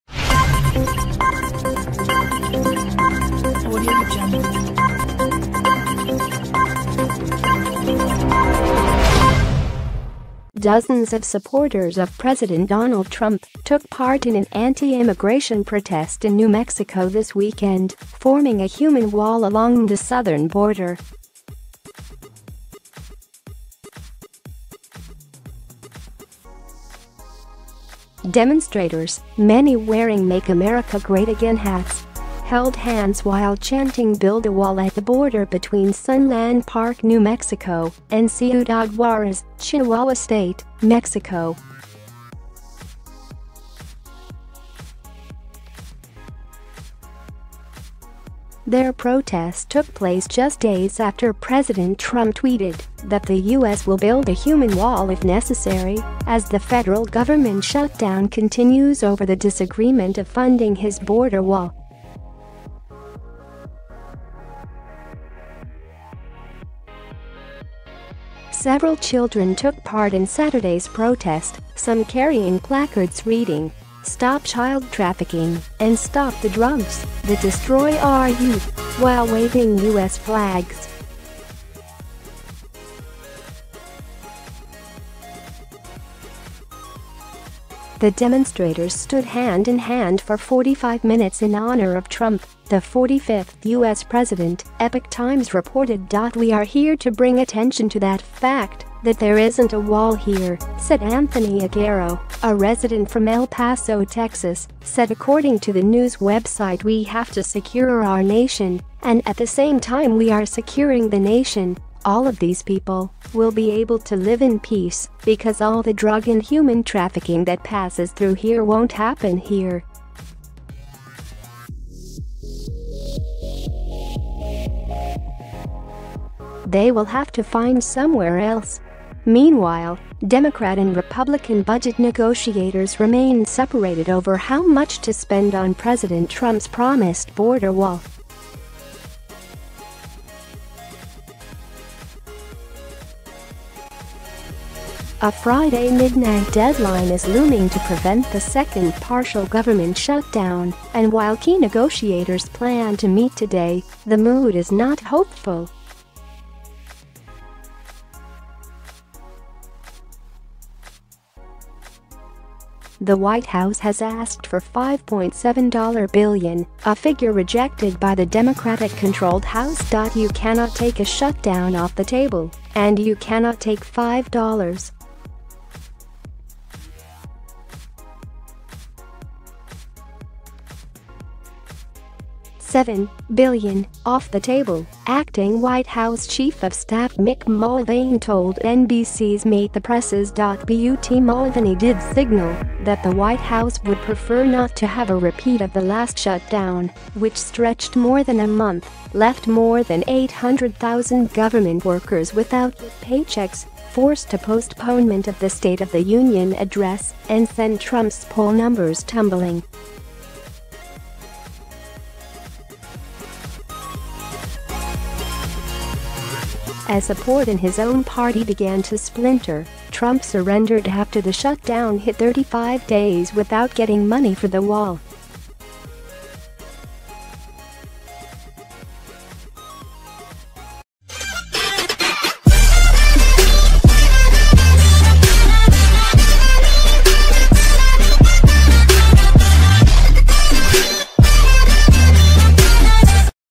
Dozens of pro-Trump supporters form 'human wall' along US border with Mexico
Dozens of supporters of President Donald Trump took part in an anti-immigration protest in New Mexico this weekend, forming a 'human wall' along the southern border. Demonstrators, many wearing 'Make America Great Again' hats, held hands while chanting 'build a wall' at the border between Sunland Park, New Mexico, and Ciudad Juarez, Chihuahua state, Mexico. Their protest took place just days after.